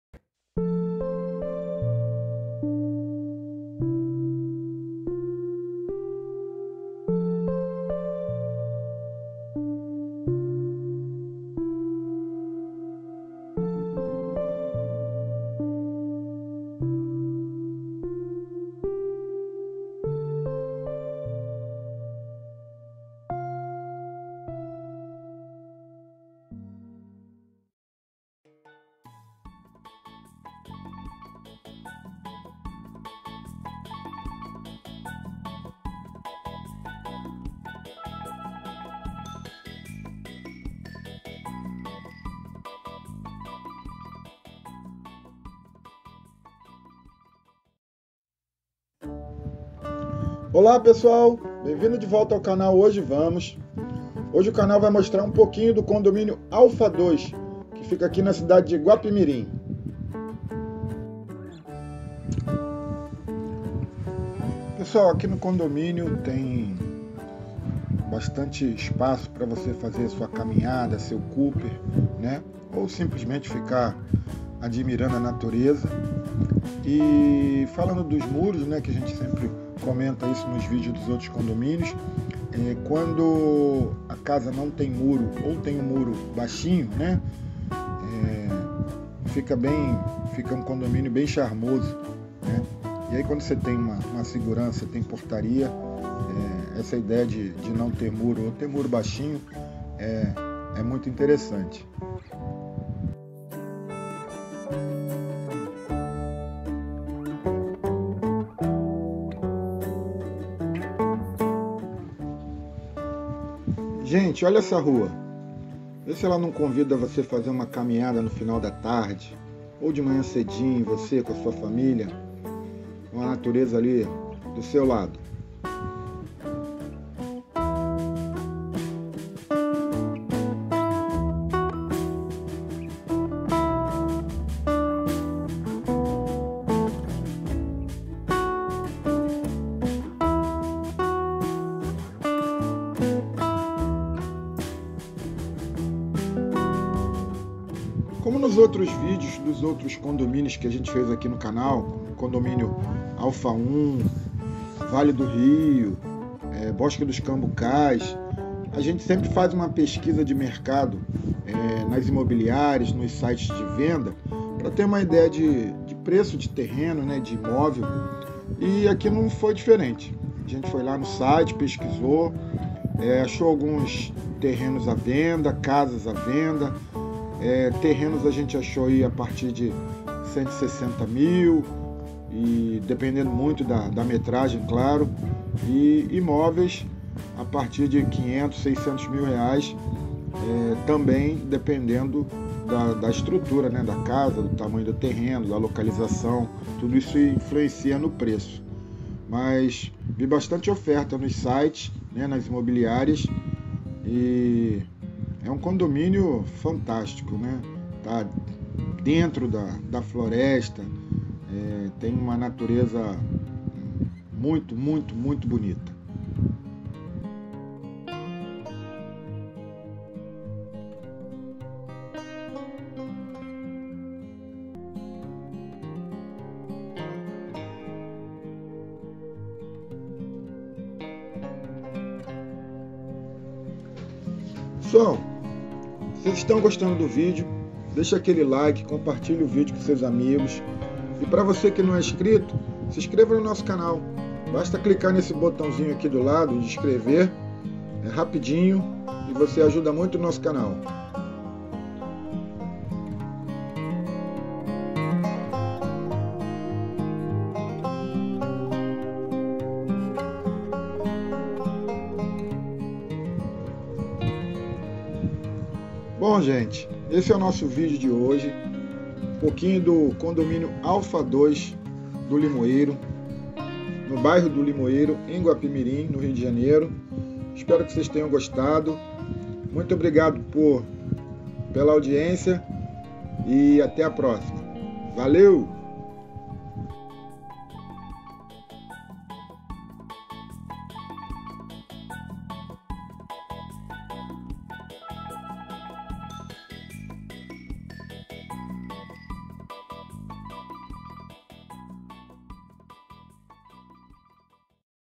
Condomínio ALPHA 2 – Guapimirim
O Condomínio Alpha 2 fica no bairro do Limoeiro na Cidade de Guapimirim. Um belo condomínio dentro da natureza.